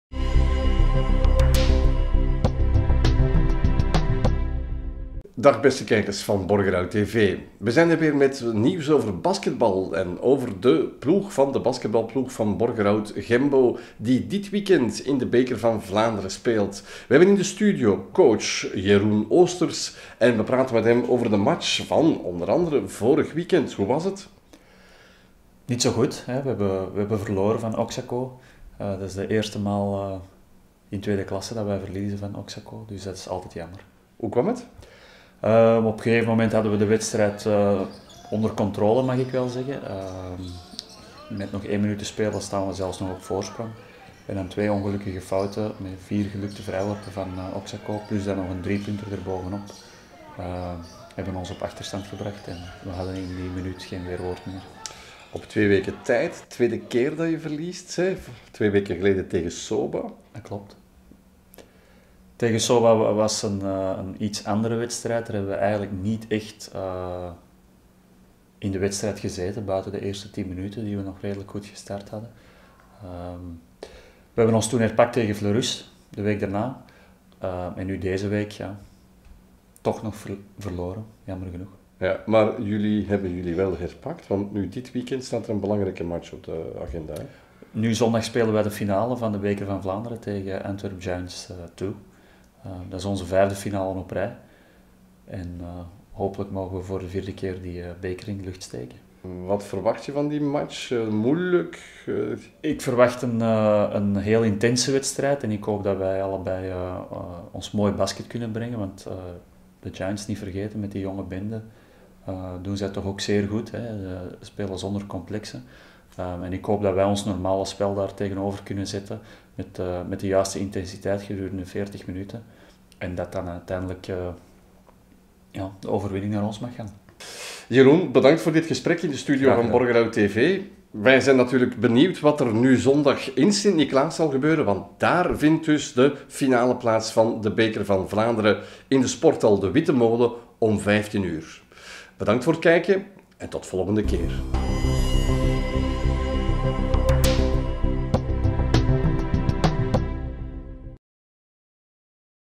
Gembo speelt finale Beker van Vlaanderen tegen Antwerp Giants2
BORGERHOUT - De Borgerhoutse basketbalploeg Gembo speelt zondag de finale van de Beker van Vlaanderen tegen de Antwerp Giants2.